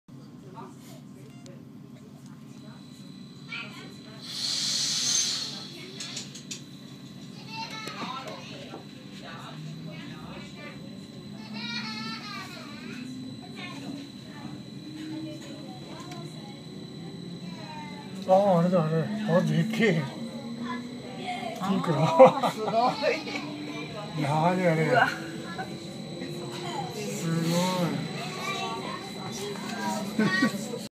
スイス・ベルン郊外のWankdorfのスイス国鉄の巨大時計【スイス情報.com】
スイス・ベルン郊外のWankdorfのスイス国鉄・SBBの巨大時計について先日投稿がありましたね。

やっと動画を撮る機会がありました。

初めて時計を見るスイス観光中のお二人のコメント付きです！

電光になっていて、不思議な時計ですね！